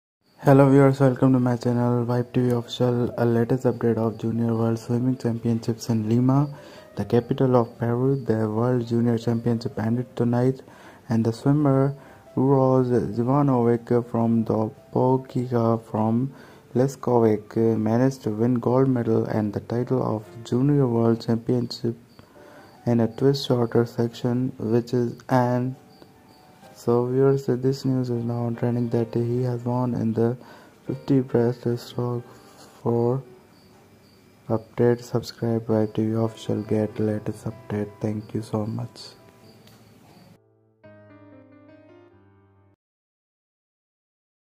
Uroš Živanović has won the Gold Medal in 50 Breaststroke at Junior Swimming World Championships 2022
In Lima, the capital of Peru, the World Junior Championship ended tonight, and the swimmer Uroš Živanović from Dubočica from Leskovac managed to win gold and the title of junior world champion in a twice shorter section, which is an unprecedented success of Serbian swimming.